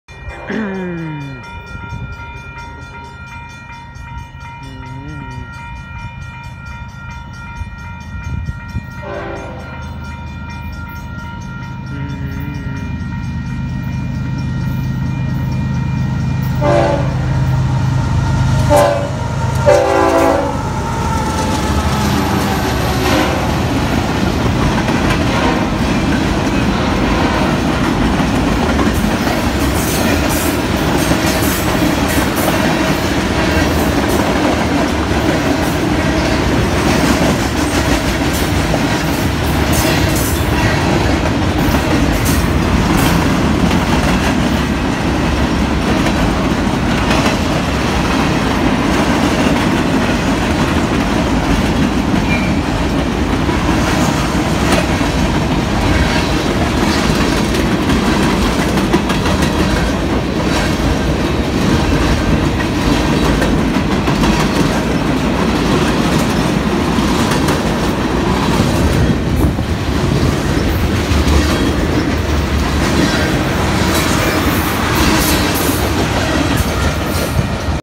Up up 7497 MWSRV going through pleasanton
Up 7497 leads the MWSRV through pleasanton